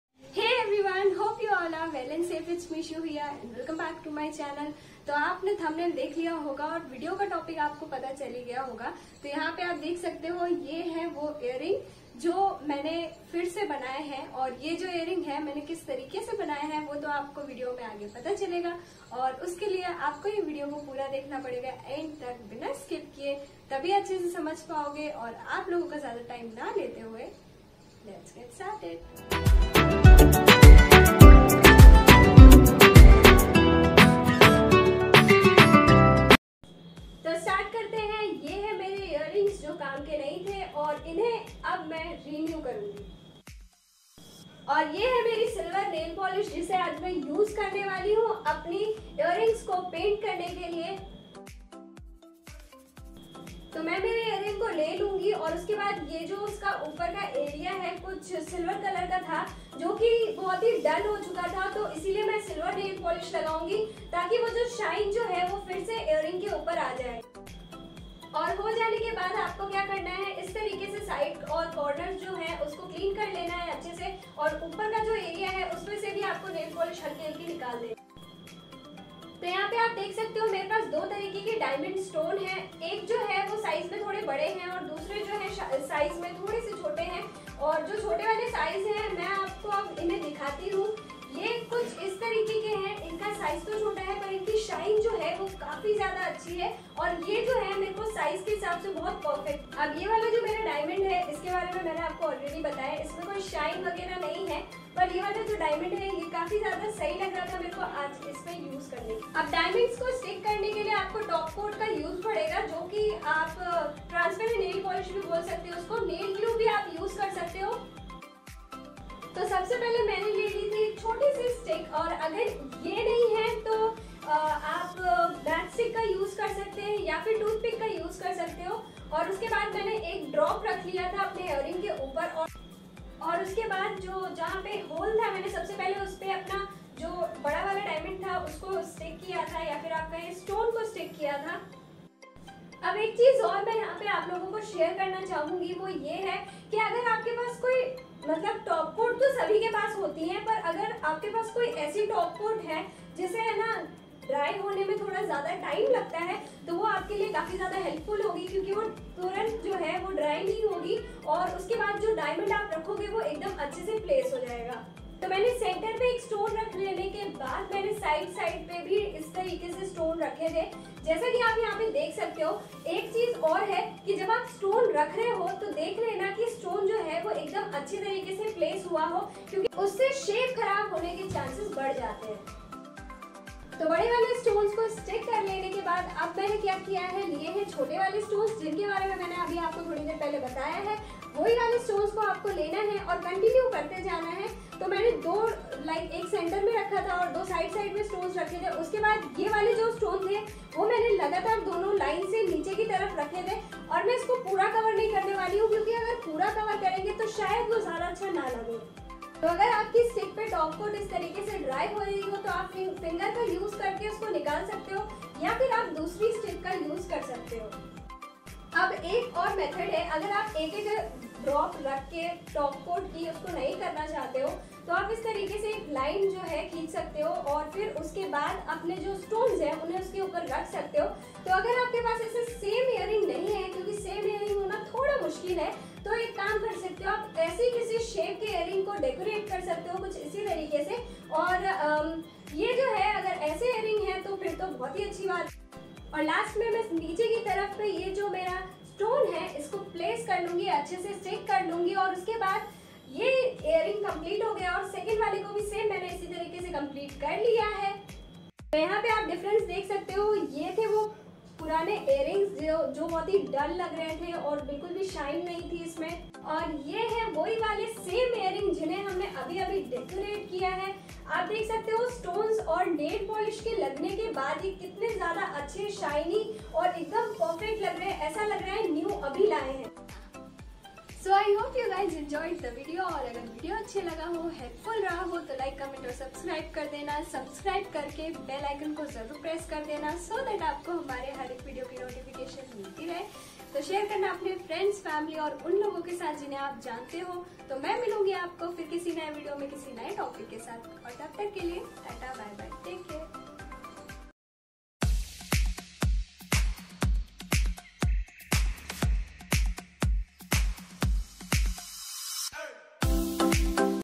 How To Renew Old Earrings | In Hindi
ABOUT VIDEO :-
Hey guys ! In today's video, i have told you how you can make make your old earrings new again by following some simple steps, It is very easy too.

                    how to renew old earrings at home
                    how to renew old earrings into new
                    homemade earring idea